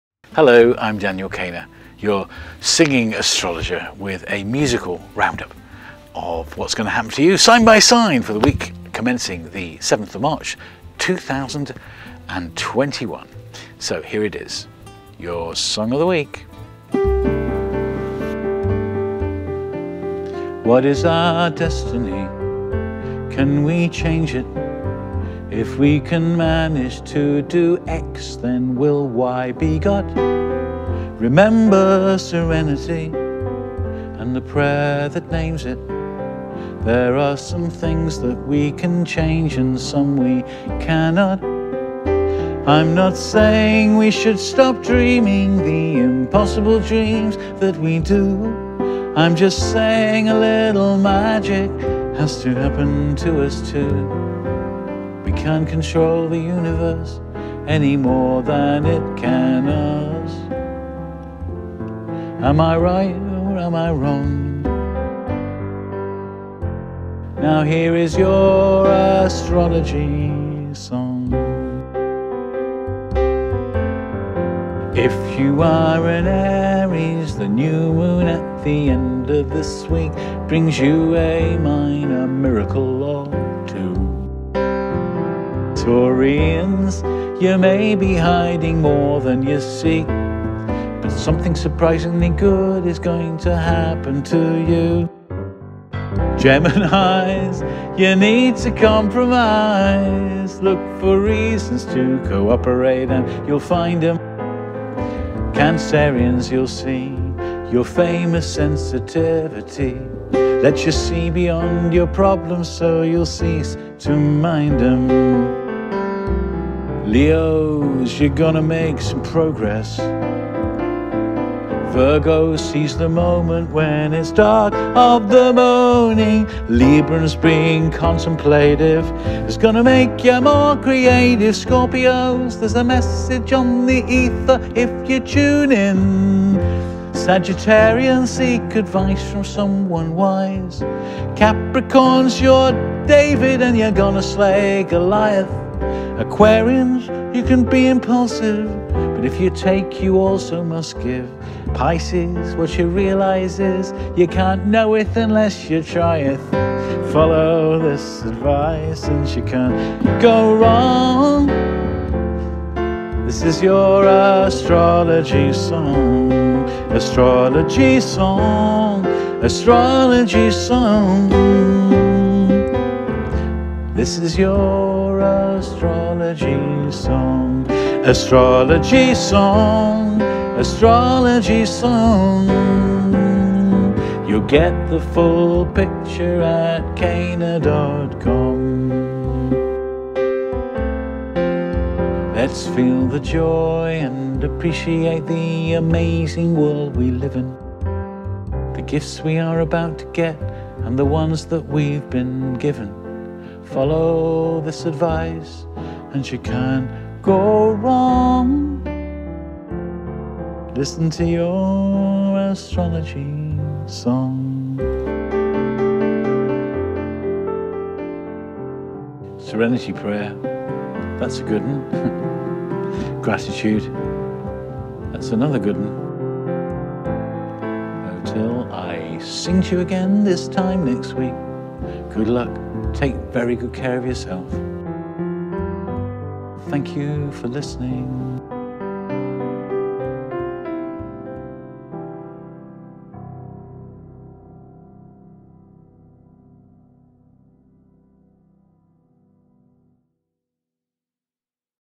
Cainer Horoscopes Song of the Week commencing 7th March 2021
Daniel Cainer presents an Astrological Song of the Week. A succinct summary for each sign.